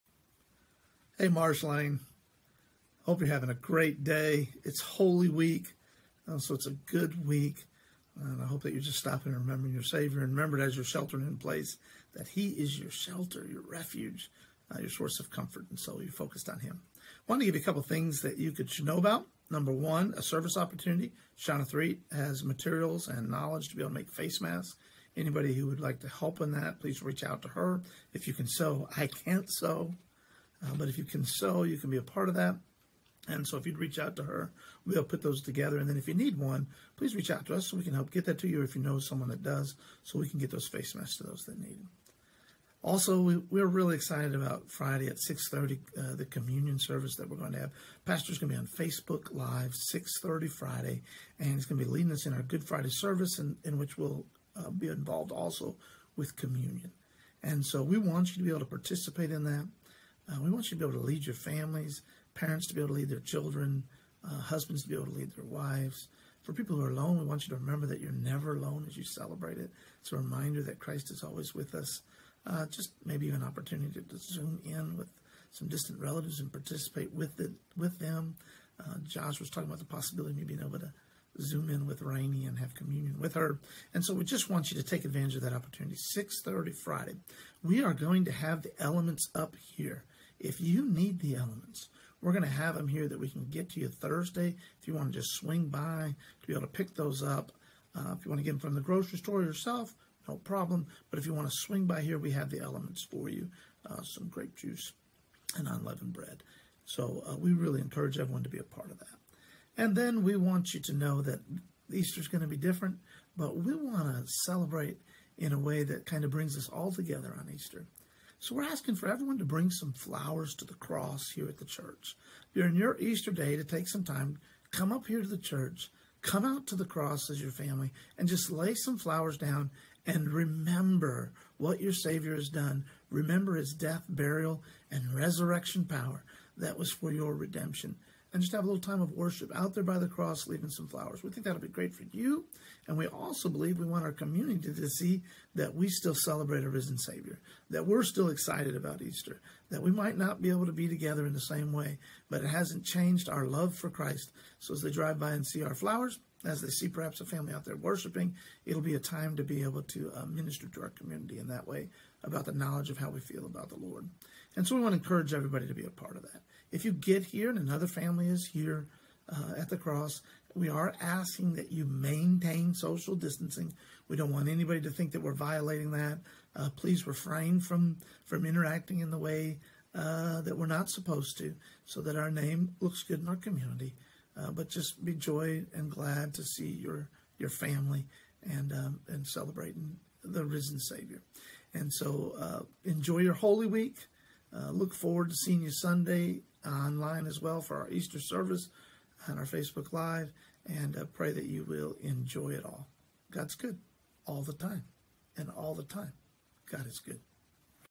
Holy Week News
Contact Shanna Threet to be a part of putting together masks for those who need them.  Don't forget about our Communion Celebration, come by the church on Thursday to pick up the elements.  For Easter, let us all visit the cross (literally), lay down flowers, have a time of prayer and gratitude for a risen Savior.  We might not be at church on Easter Sunday, but we have not forgotten the cross.